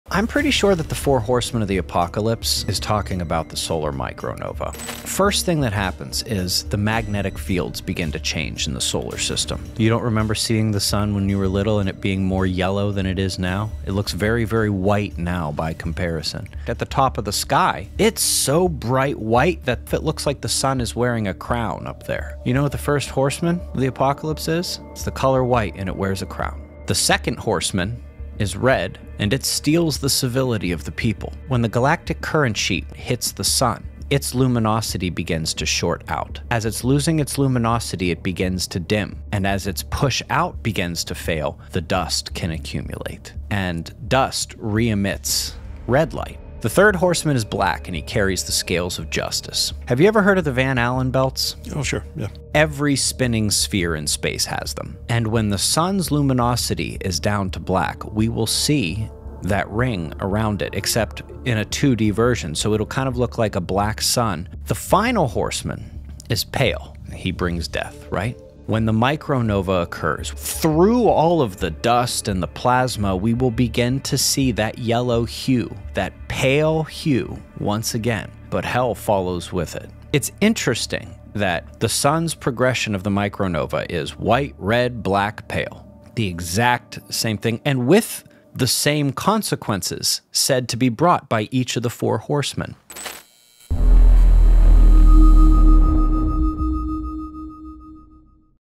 A Solar Link to the Four Horsemen of the Apocalypse?
Ben from @SpaceWeatherNewsS0s  shares his interpretation of the Four Horsemen of the Apocalypse as a symbolic description of solar behavior.

He connects the colors…white, red, black, and pale…to stages he associates with a solar micro nova event: shifts in luminosity, magnetic change, and visible dust accumulation.

His perspective ties ancient symbolism to cosmic patterns, blending mythology and astrophysical theory into one idea.